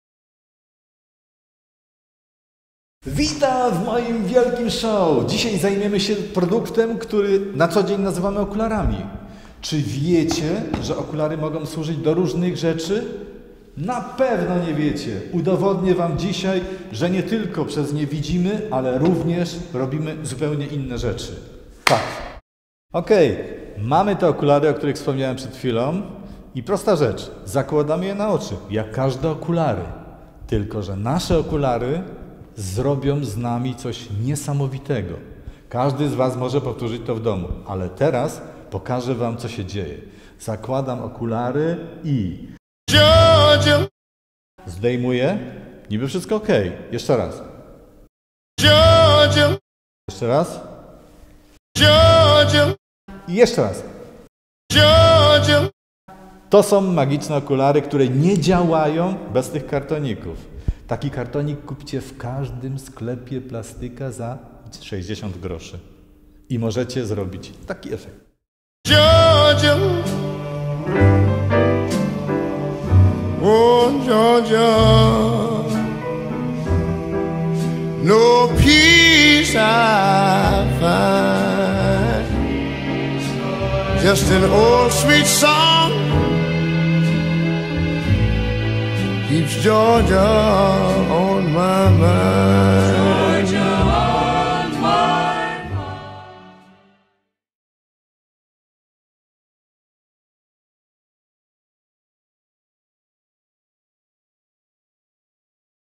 foto Ray Charles by Norman Seeff
Ożywienie dzieła sztuki - na podstawie fotografii portretu Raya Charlesa wykonanej przez Normana Seeffa w Los Angeles w 1985 roku. Film jest pracą semestralną na ASP we Wrocławiu.